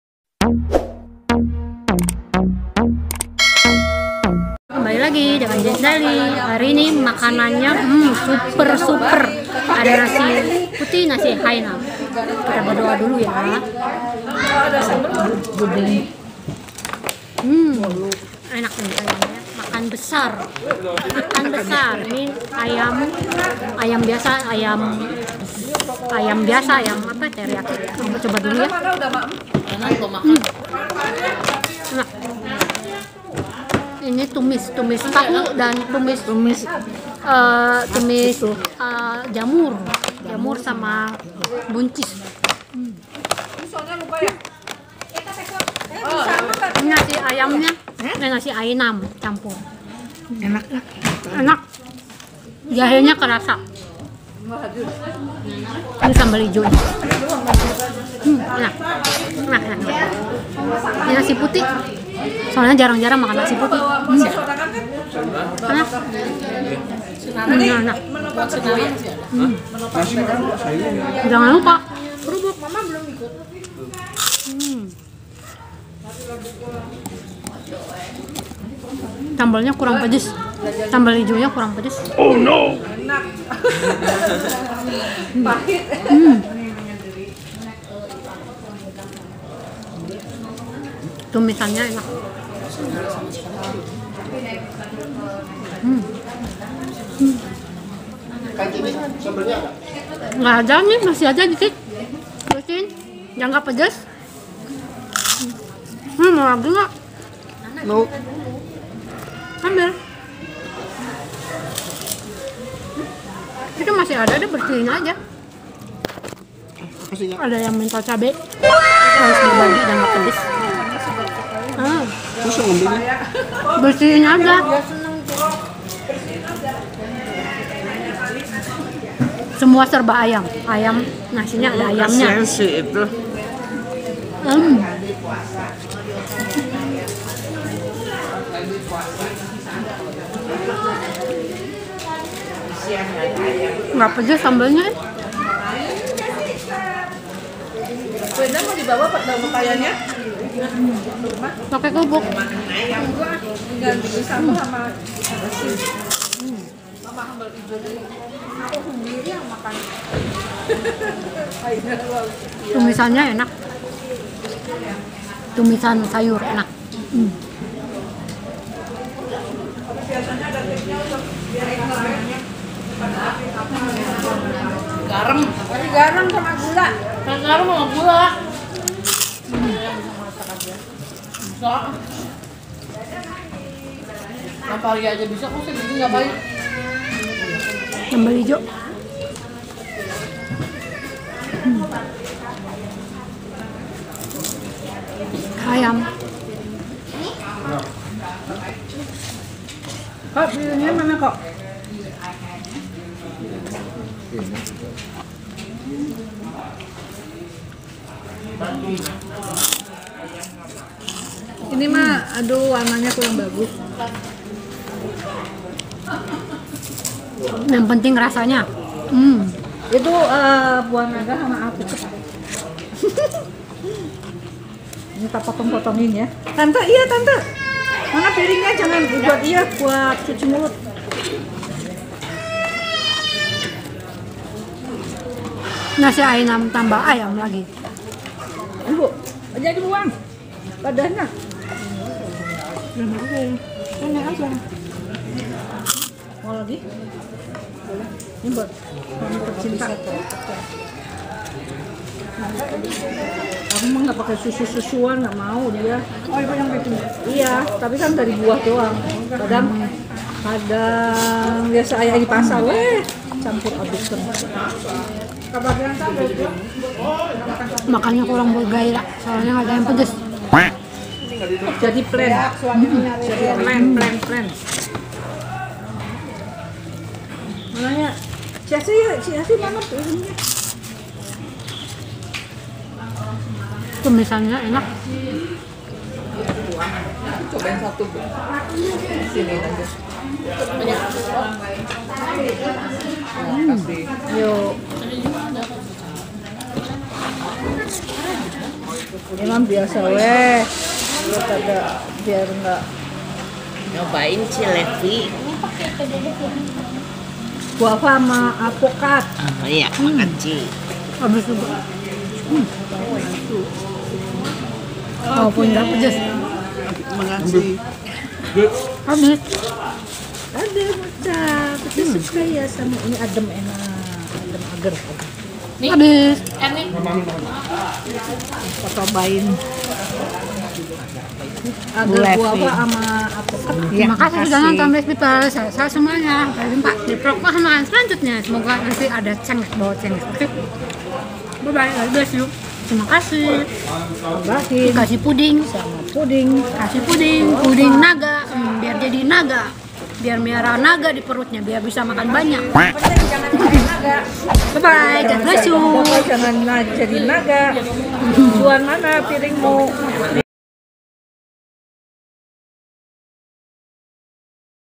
📍MUKBANG SERBA AYAM, SUPER YUMMO, TAPI SAYANG GA PEDAS 🤭 @JennsDaily ⁉️
📍MUKBANG SERBA AYAM, SUPER YUMMO, TAPI SAYANG GA PEDAS 🤭 ⁉️ Mukbang Nasi Hainam ‼️ Asmr Chinese Food !! Jenn'sdaily

Hey blessed people 👋👋
Mari makan lagi selagi ada nafsu untuk makan 😁🤭😋
Hari ini @Jenn'sDaily maw berbagi tips Simple buat kalian yang maw menurunkan Berat Badan, Dan yang mau menjaga berat badan agar stabil.

Ada 5 Tips yang maw @Jenn'sDaily Share buat blessed people semuanya, Cara ini akan berguna jika Dilakukan dengan KONSISTEN..!!!
Ini sudah berhasil Aku praktekin ya blessed people semoga berhasil buat blessed viewer semuanya..!

1. Minum 1 gelas air putih sebelum makan.
2. Pilih Piring makan yang kecil (Porsi Kecil)
3. Jam makan jangan berubah-ubah
4. Kunyah makanan 22-32 kunyahan
5. Bawa Bakal Siang Sendiri

video mukbang asmr#@Jenn'sDaily